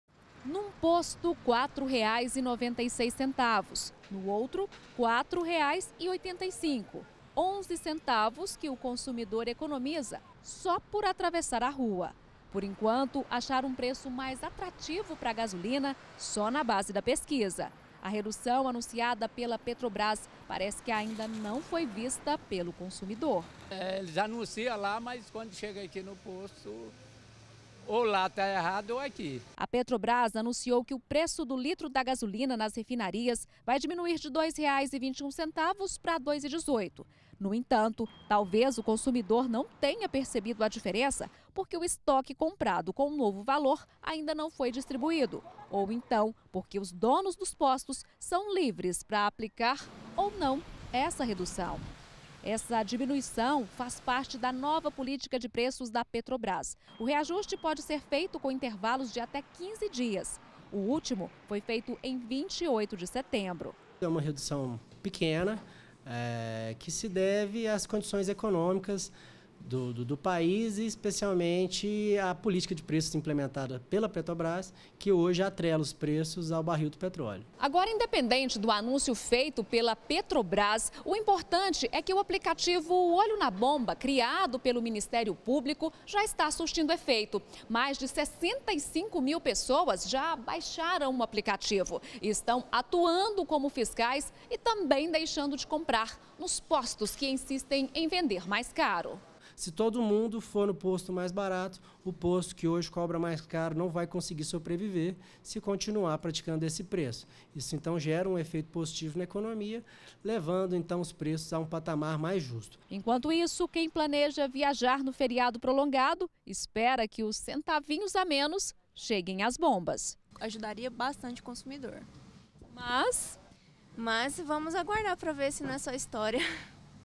GR - Gasolina mais barata: consumidor não sente diferença de preço - 09-10-2018
A Petrobrás anunciou que a gasolina vai ficar barata. Nas refinarias. E pouca coisa, 3 centavos por litro. Boa hora pra botar em prática um aplicativo que ajuda o consumidor a buscar combustível mais barato.